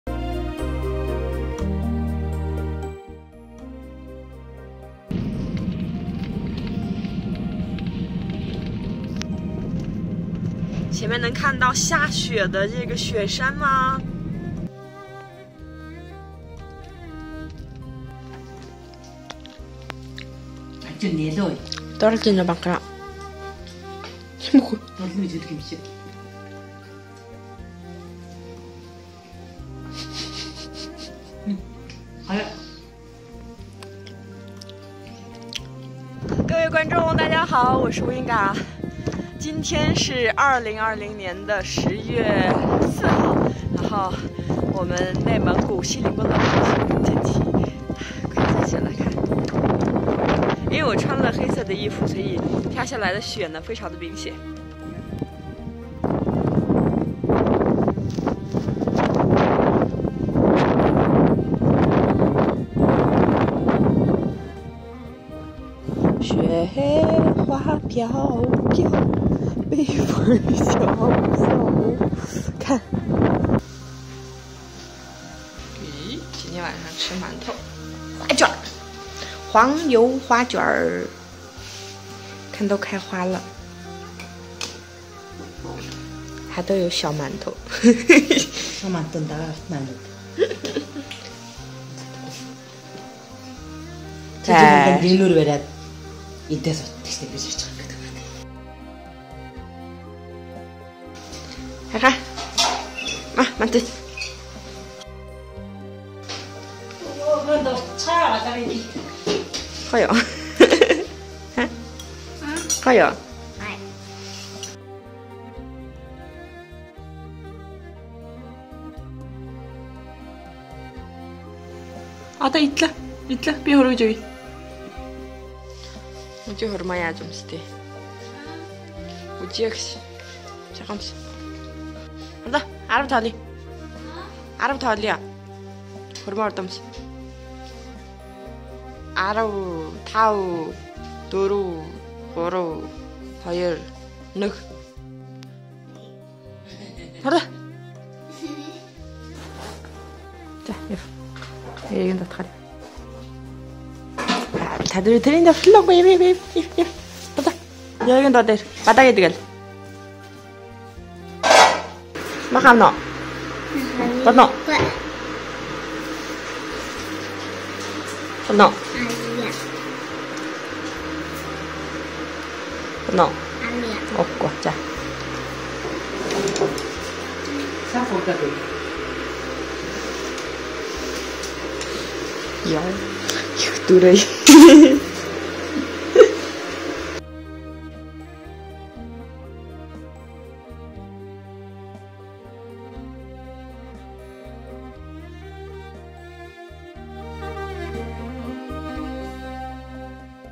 第二季(28) 草原十月下雪羽绒服上身 南北地区差异太大 冬天怎么办？
乌音嘎 第二季(28) 草原十月下雪羽绒服上身 南北地区差异太大 冬天怎么办？

(1028) 草原十月下雪羽绒服上身 南北地区差异太大 冬天怎么办？

草原十月飞雪？大衣羽绒服上身？看牧区晚餐的黄油花卷配肉汤！